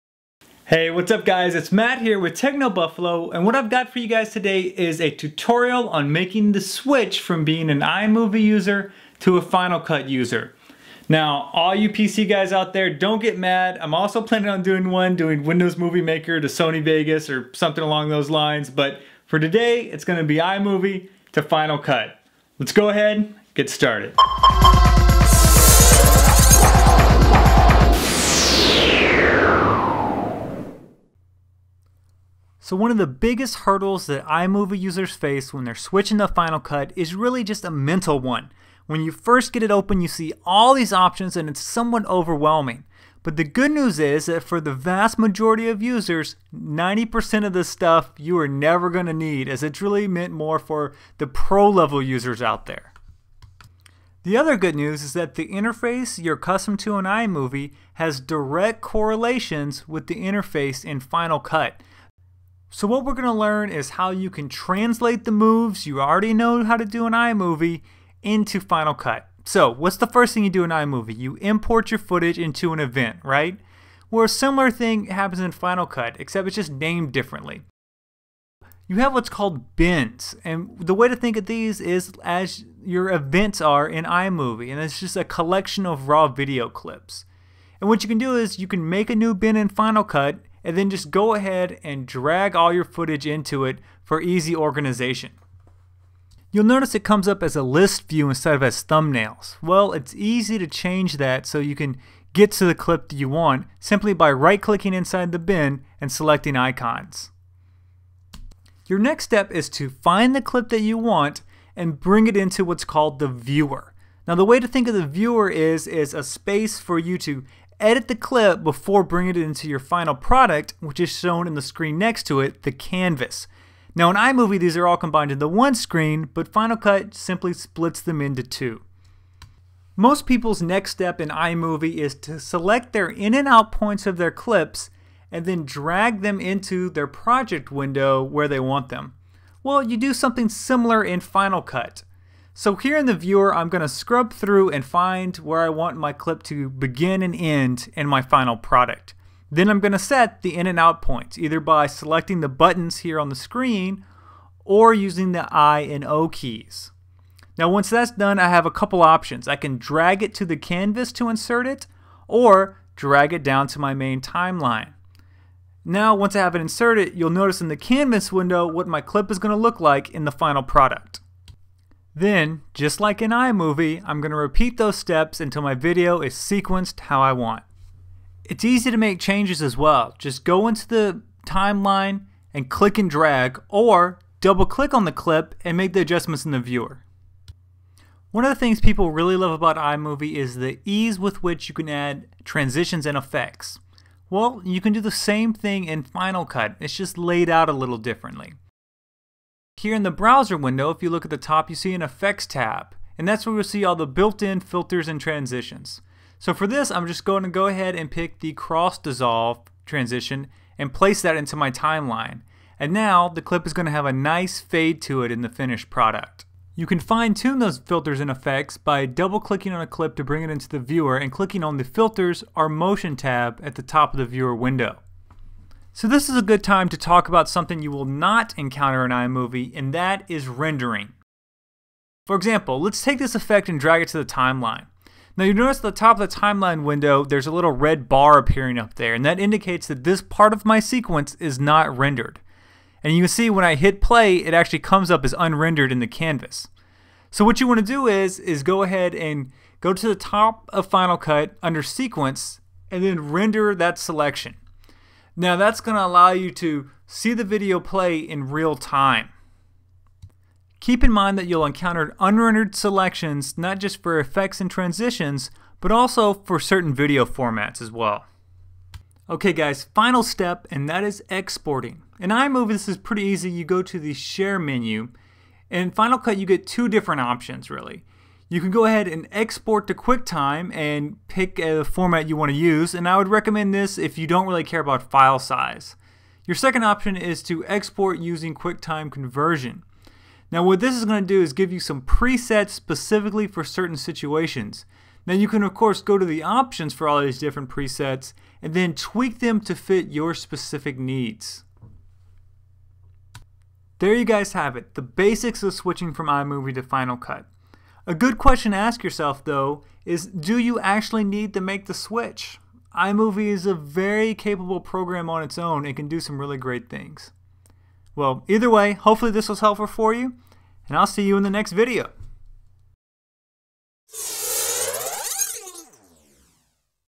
iMovie to Final Cut: Upgrade Made Easy
If you are a video creator looking for a bit more power than iMovie can provide then Final Cut (or Express) may be just what you are looking for.  In this video we walk users through the basics and show them how to import, edit, and export their final films.

iMovie to Final Cut Upgrade